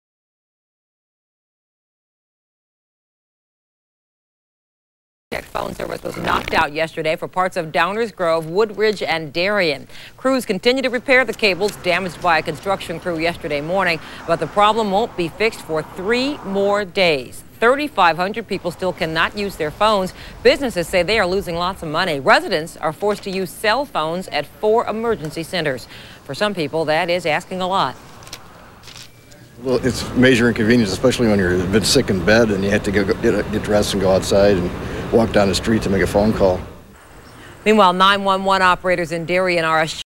Ameritech Phone Outage 1990's
Major cable cut in Dupage County Illinois. Phone men work overtime